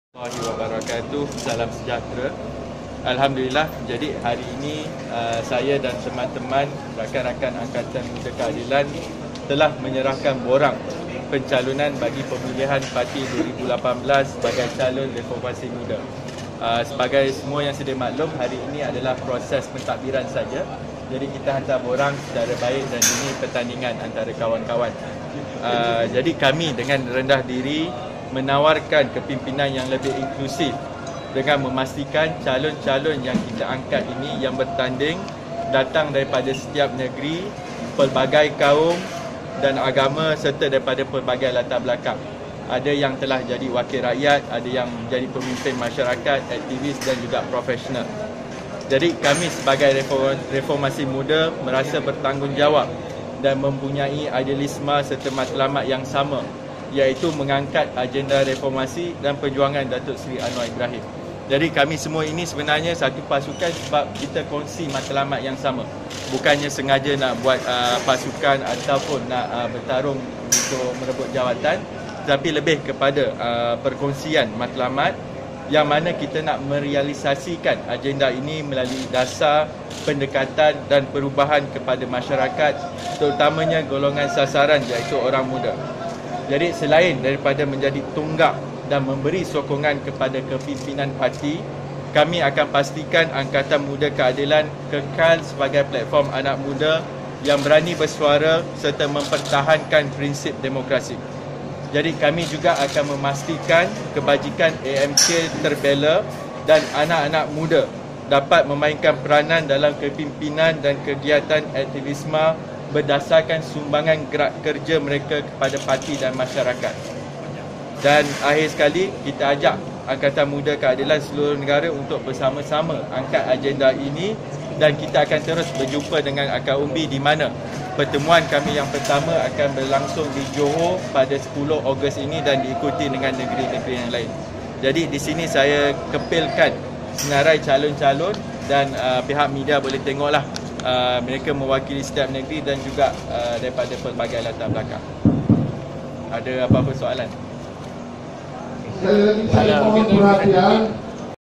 公正黨黨選‧阿克馬提名 角逐公青團長職
（本報馬錦源攝）